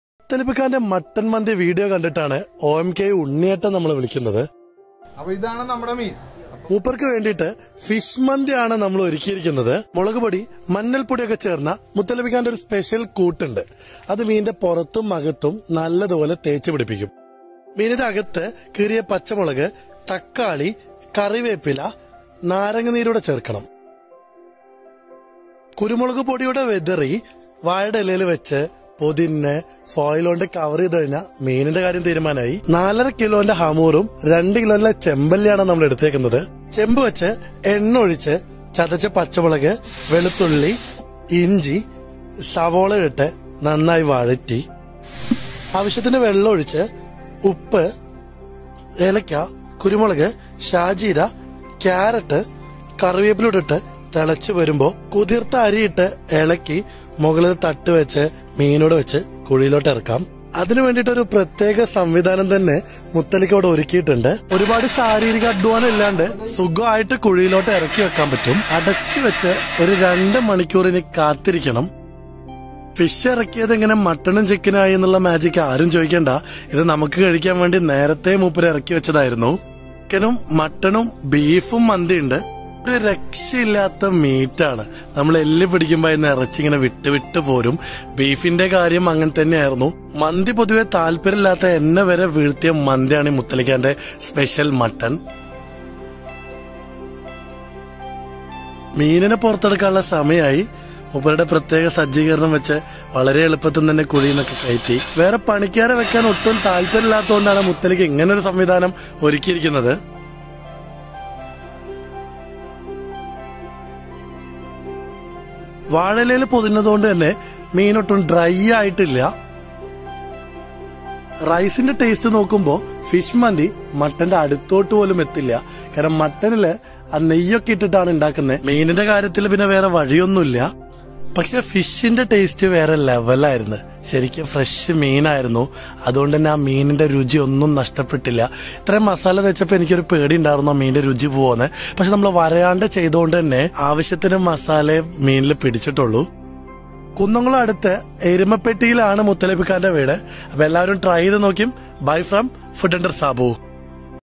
Fish Mandhi Recipe | OMKV ഉണ്ണിയേട്ടനോടൊപ്പം മുത്തലിബിക്കാടെ ഫിഷ് മന്തി
കുറച്ച് നാൾ മുൻപ് ത്രിശൂർ എരുമപ്പെട്ടിയിലെ മുത്തിക്കാടെ രസിപ്പി വച്ച് ചെയ്ത വീഡിയ ആണ് ഇന്നത്തേത്.@OMKVFishingCooking  ഉണ്ണിയേട്ടനും നമുക്കൊപ്പമുണ്ടായിരുന്നു.